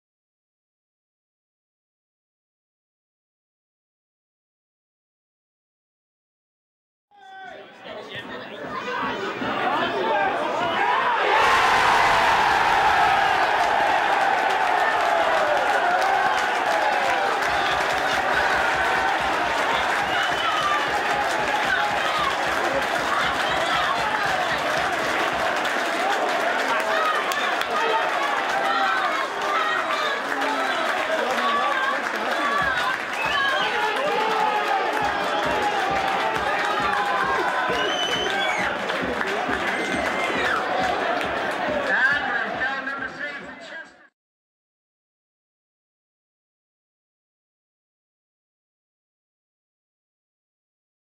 Ross Killock's goal for Chester FC against Nuneaton Town
Chester take a 3-2 lead through Ross Killock at the Swansway Chester Stadium in what turns out a 3-3 draw in the Skrill Premier match. Thanks to NATV for the footage.